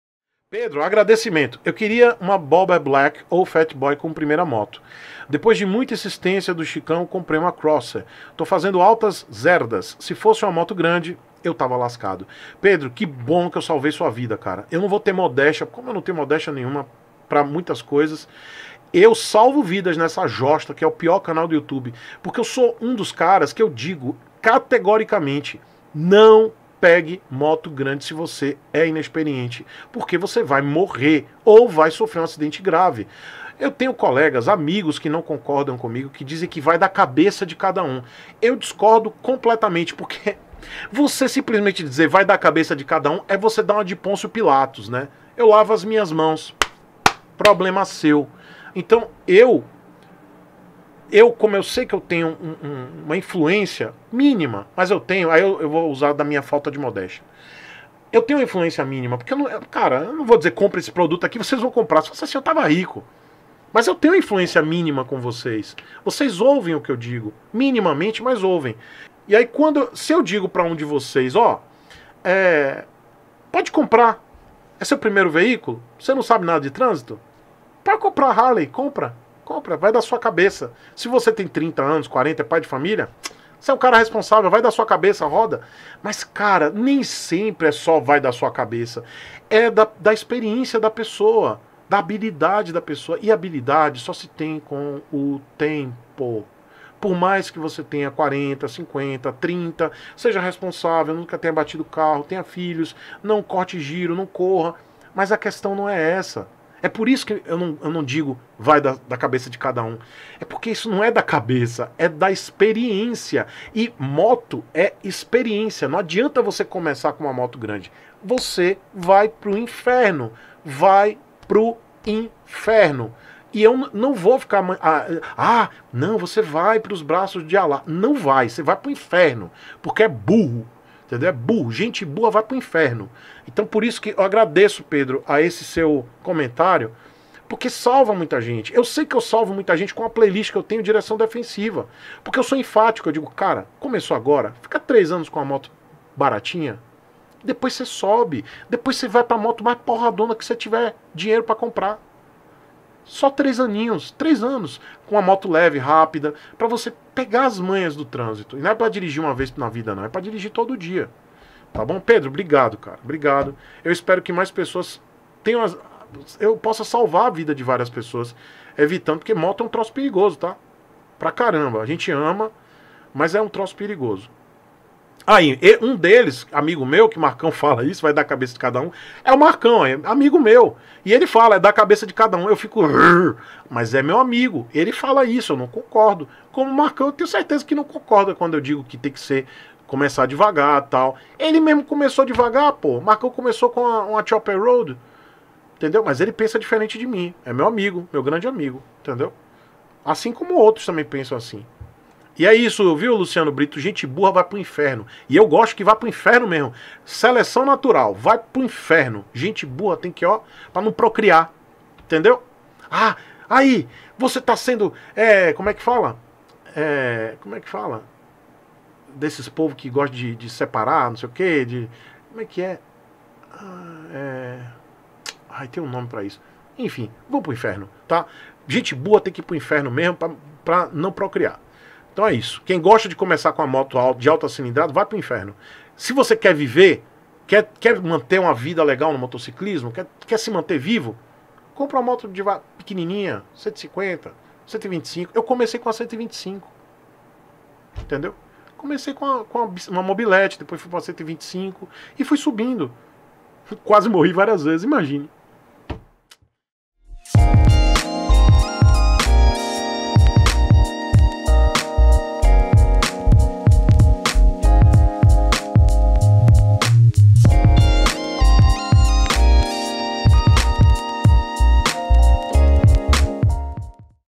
Por que NÃO RECOMENDO motos de ALTA CILINDRADA para INICIANTES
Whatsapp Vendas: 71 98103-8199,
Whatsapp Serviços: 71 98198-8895